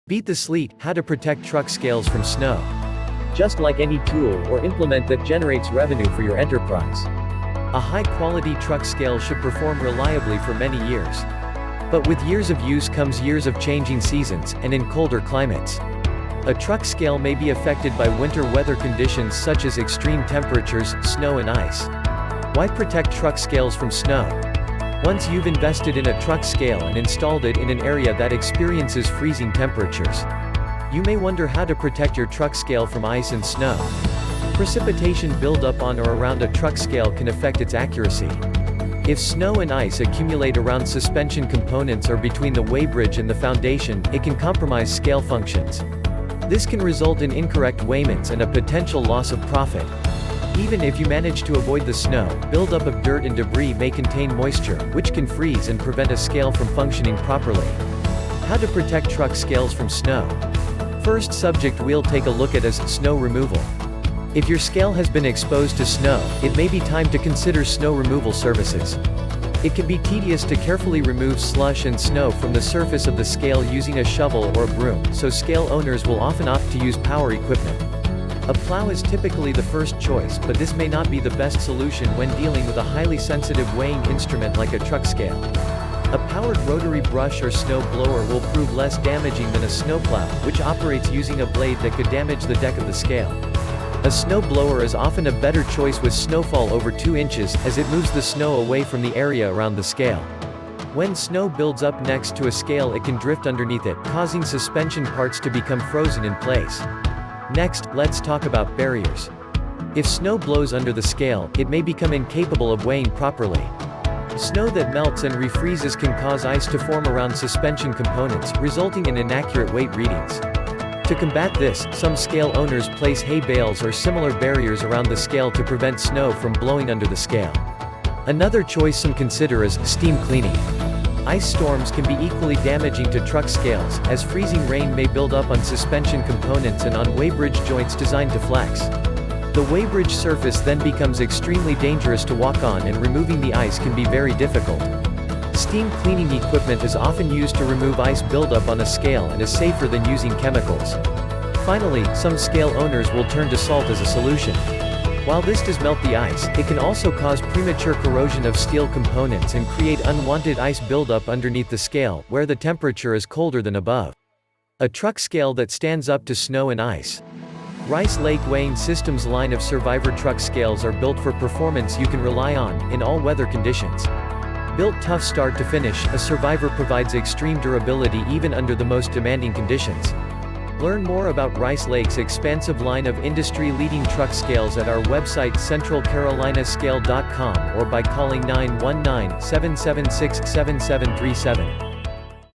How to Remove Snow from a Truck Scale
Just like any tool or implement that generates revenue for your enterprise, a high-quality truck scale should perform reliably for many years.

But with years of use comes years of changing seasons, and in colder climates, a truck scale may be affected by winter weather conditions such as extreme temperatures, snow and ice.

Once you’ve invested in a truck scale and installed it in an area that experiences freezing temperatures, you may wonder how to protect your truck scale from ice and snow.

Precipitation buildup on or around a truck scale can affect its accuracy. If snow and ice accumulate around suspension components or between the weighbridge and the foundation, it can compromise scale functions.

This can result in incorrect weighments and a potential loss of profit.

Even if you manage to avoid the snow, buildup of dirt and debris may contain moisture, which can freeze and prevent a scale from functioning properly.

How to Protect Truck Scales from Snow
If your scale has been exposed to snow, it may be time to consider snow removal services. It can be tedious to carefully remove slush and snow from the surface of the scale using a shovel or a broom, so scale owners will often opt to use power equipment.

A plow is typically the first choice, but this may not be the best solution when dealing with a highly sensitive weighing instrument like a truck scale.

A powered rotary brush or snow blower will prove less damaging than a snowplow, which operates using a blade that could damage the deck of the scale. A snow blower is often a better choice with snowfall over 2 inches, as it moves the snow away from the area around the scale.

When snow builds up next to a scale it can drift underneath it, causing suspension parts to become frozen in place.

If snow blows under the scale, it may become incapable of weighing properly. Snow that melts and refreezes can cause ice to form around suspension components, resulting in inaccurate weight readings. To combat this, some scale owners place hay bales or similar barriers around the scale to prevent snow from blowing under the scale.

Ice storms can be equally damaging to truck scales, as freezing rain may build up on suspension components and on weighbridge joints designed to flex. The weighbridge surface then becomes extremely dangerous to walk on– and removing the ice can be very difficult. Steam-cleaning equipment is often used to remove ice buildup on a scale and is safer than using chemicals.

Some scale owners will turn to salt as a solution. While this does melt the ice, it can also cause premature corrosion of steel components and create unwanted ice buildup underneath the scale, where the temperature is colder than above.